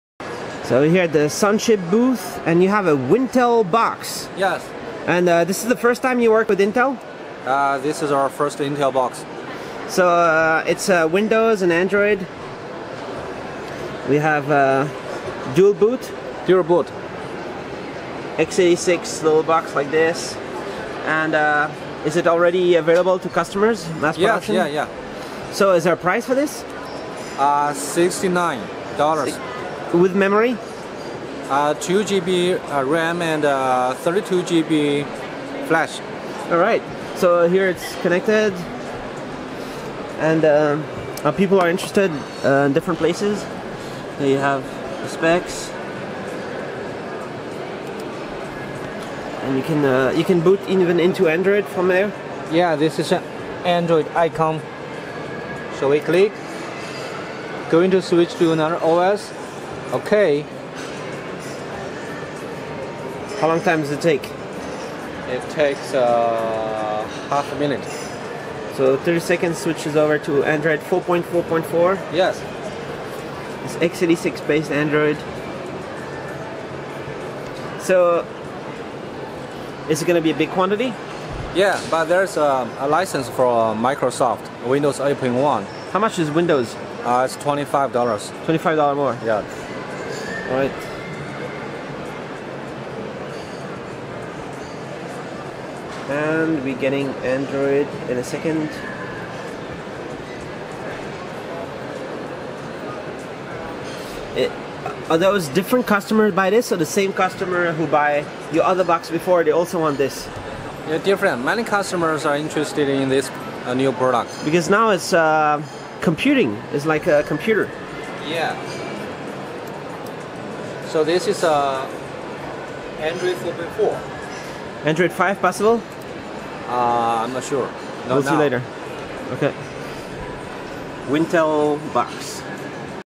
$69 Sunchip Wintel Box CX-W8 on Intel Windows/Android Dual-boot
Sunchip releases their first Intel based Wintel box the CX-W8, running Intel x86 Z3735F with 2GB RAM and 32GB Flash, selling starting at $69 for 1 thousand pieces without the $25 Windows 8.1 Licence.

You can contact Sunchip here:
Shenzhen Sunchip Technology Co., Ltd
Kobe Chen, Sales Manager
Skype: Kobechen6132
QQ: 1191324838